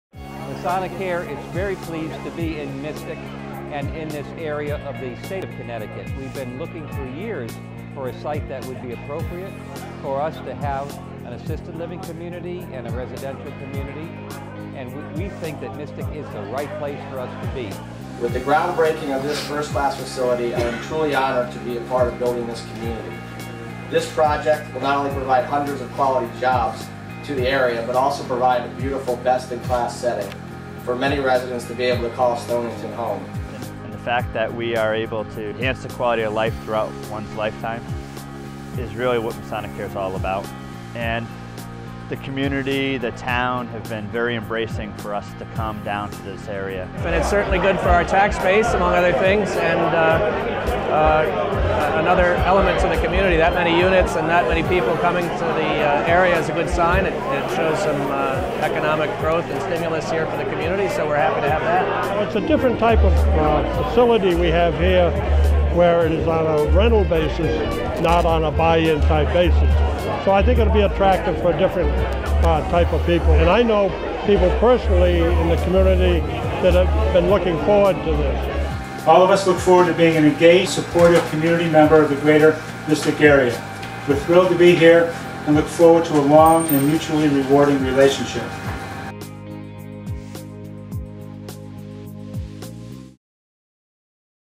Masonicare at Mystic Groundbreaking Ceremony
Clips from the May 22 groundbreaking ceremony for our new senior living community in Mystic, CT.